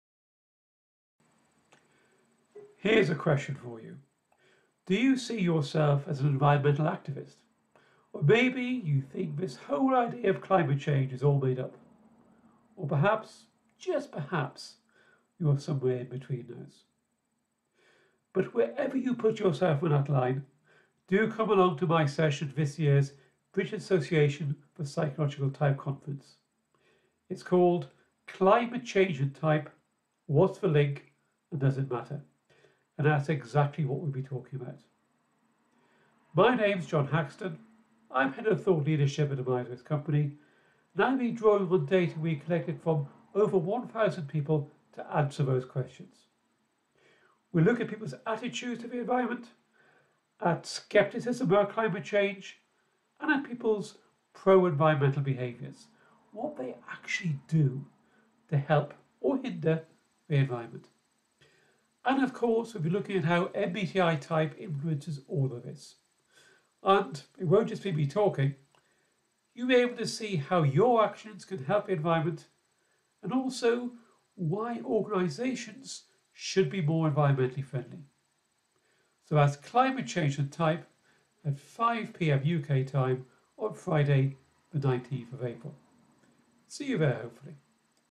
Climate Change and Personality Type - What's the Link and Does It Matter? // John Hackston (Trailer)
This session will take place during our virtual conference on 19-20 April 2024, with the theme: Personality - Let's Talk About It. Conversations in a Complex World.
We invite you to join us at BAPT2024!

SESSION AT A GLANCE
This research-based interactive session explores how type and other factors relate to
individual and organizational green behaviour and beliefs. You'll evaluate how sustainably you
and your organization operate, and practise using type to communicate green issues.